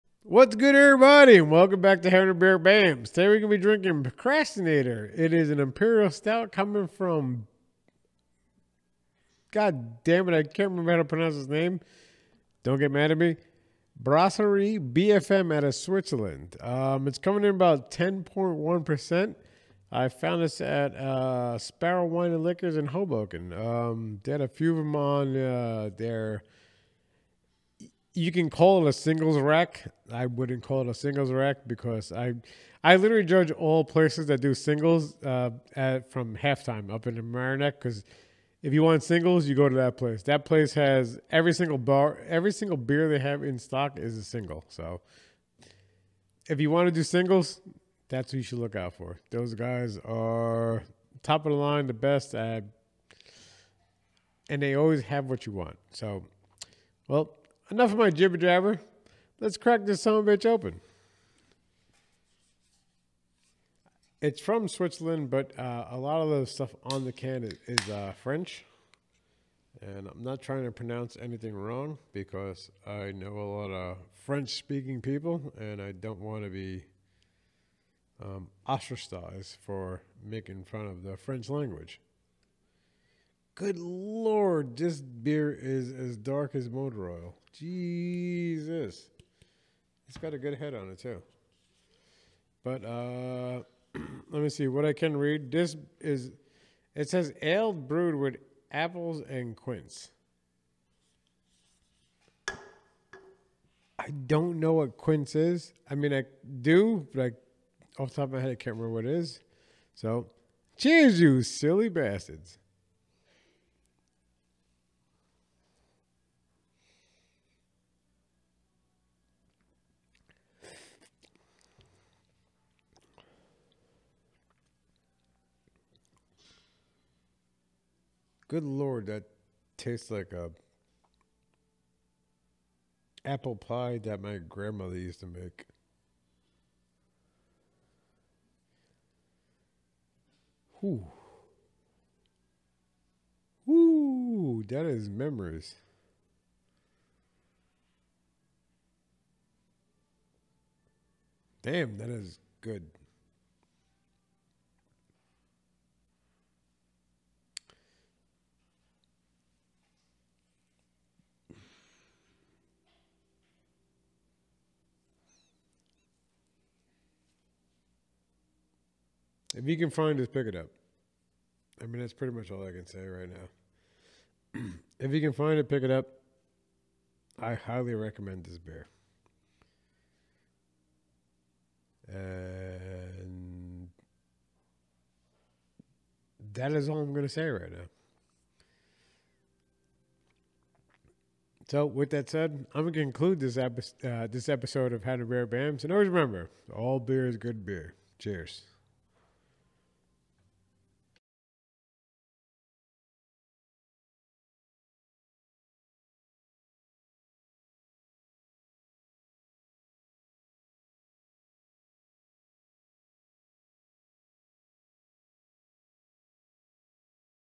The Silly Bastard has a flash back.
Episode:  Having Procrastinator 4 from Brasserie des Franches-Montagnes (BFM) with Bamms.

Gear Used:
Lighting: Godox VL 150 with a P120L Parabolic Softbox
2 Dracast Camlux Pro Bi-Color LED panels
Audio:  Zoom Livetrak 8